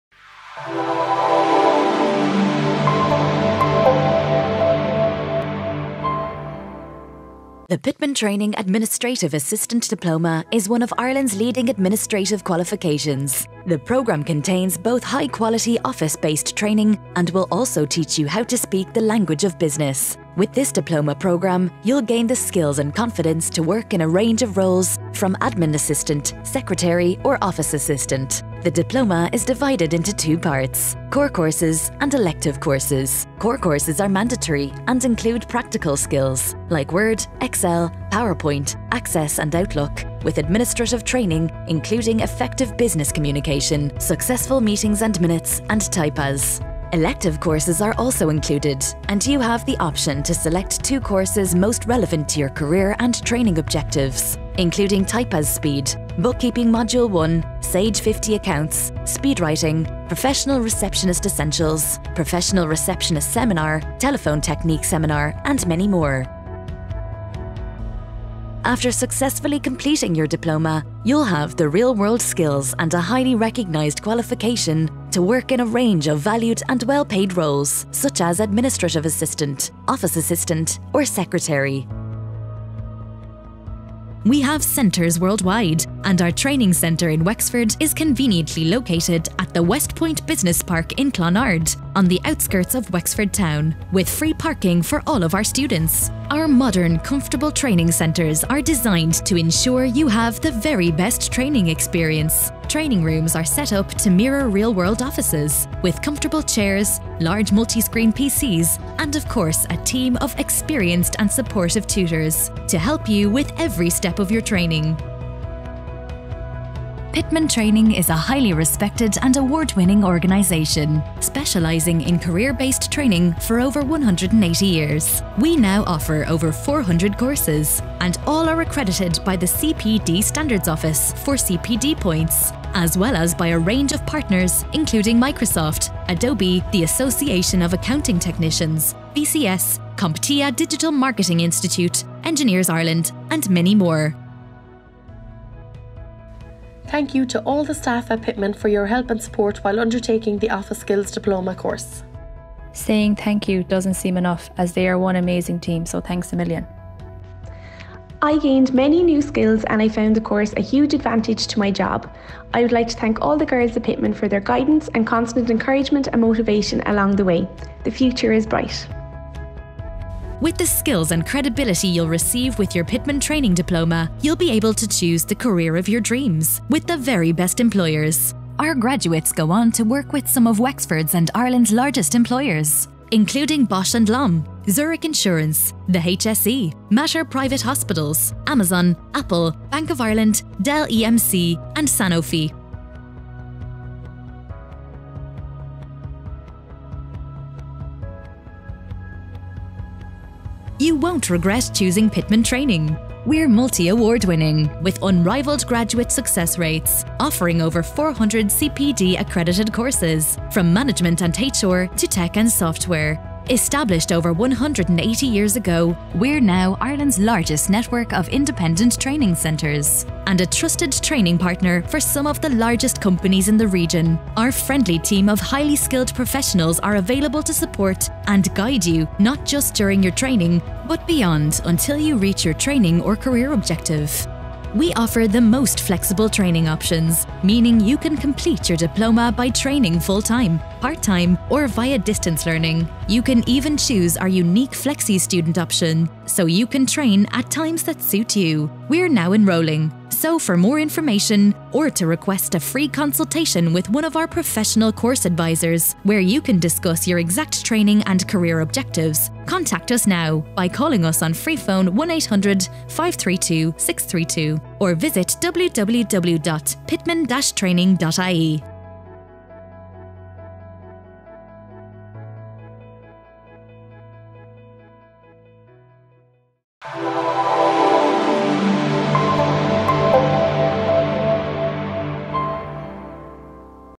Administrative Assistant Courses Wexford 2020
The Pitman Training Administrative Assistant Diploma is one of Ireland's leading Administrative qualifications.

The programme combines both high quality office based training and will also teach your how to speak the language of business.
With this Diploma programme you'll gain the skills and confidence to work in a range of roles from Admin Assistant, Secretary or Office Assistant in Wexford and surrounding areas.

For more information chat to one of our professionally trained Course Advisors and Tutors in our Wexford Training Centre.